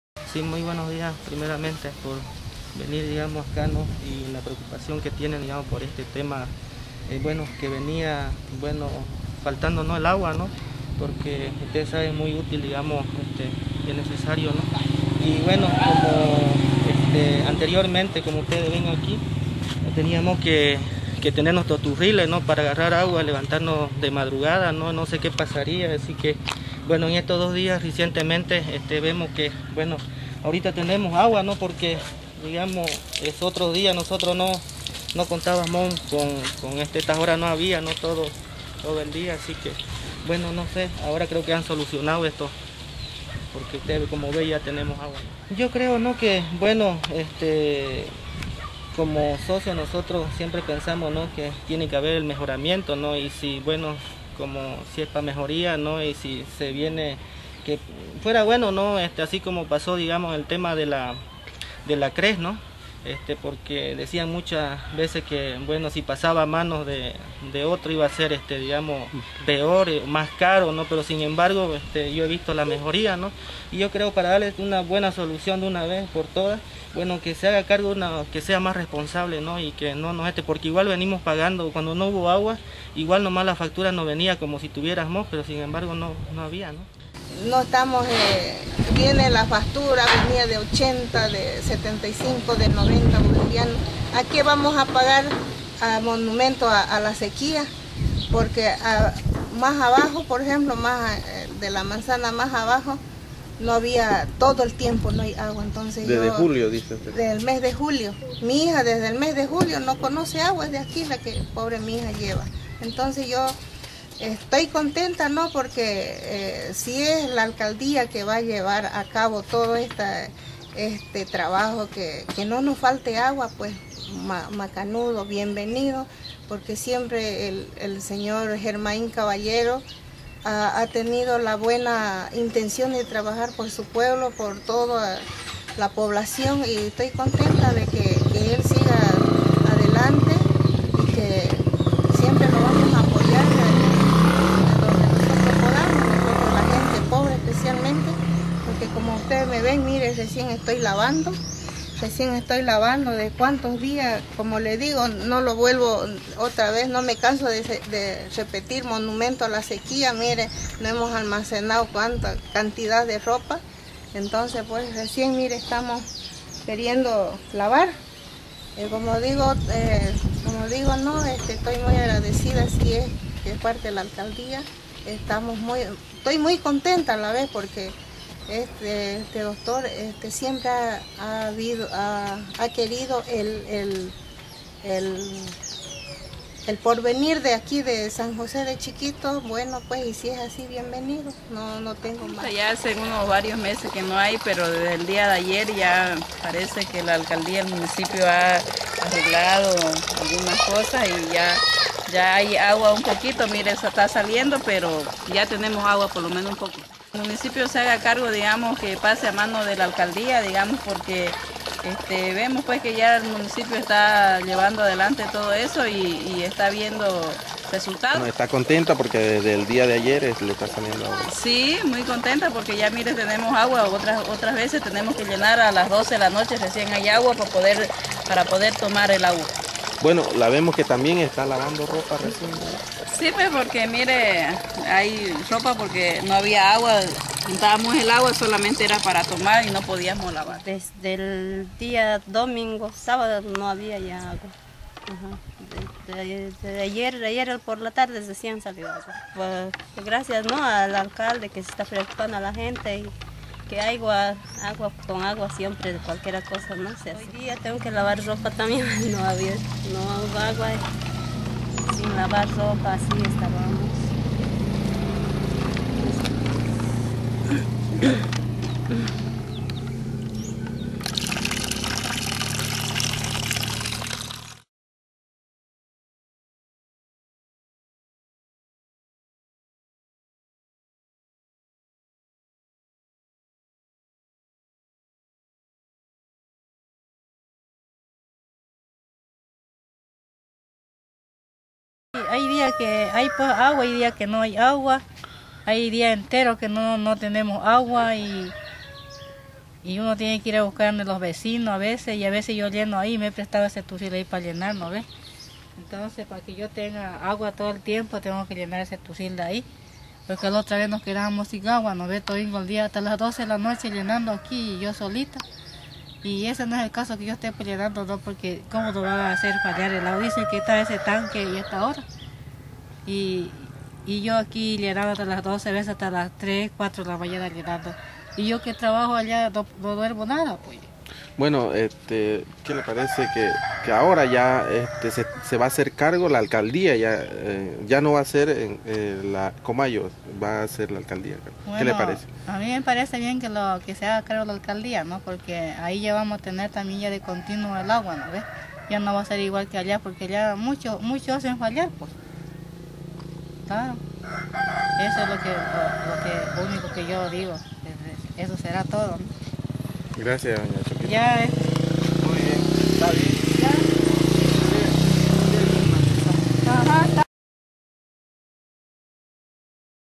opiniones agua
Problemas de Agua en San Jose es muy comun, pero ya no se aguantaba mas y la Alcaldia Interviene la Cooperativa COMAYO, como una manera de salvar la situacion tan dificil en esta epoca seca, pudimos capturar lgunas opinines luego de algunos trabajos por parte de los tecnicos de la Alcaldia en cambio de bombas en los pozos de agua y otras mejoras mas.